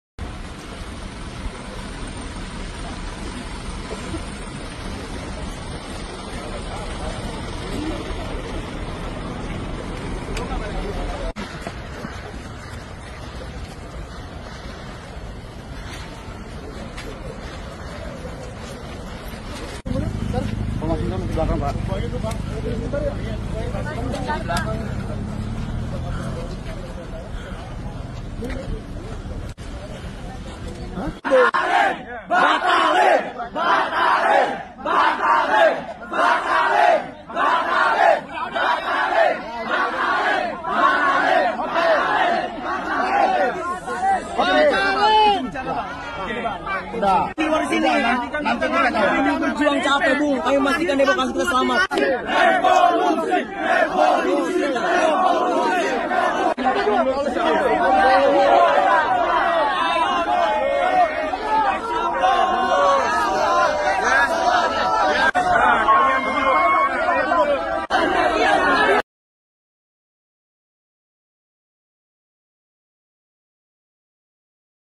Detik-detik Wakil Rakyat bertemu Rakyat
Sejumlah Anggota Badan Legislasi (Baleg) Dewan Perwakilan Rakyat (DPR) RI tampak menemui mahasiswa ketika aksi unjuk rasa di depan Gedung DPR RI, Kamis malam, 22 Agustus 2024.
Seperti dalam video yang diunggah akun Instagram pribadi @putranababan74.
Ia tampak bersama @arteriadahlan @masinton menerima aspirasi rakyat Indonesia yg disuarakan mahasiswa agar DPR RI konsisten menjalankan putusan MK.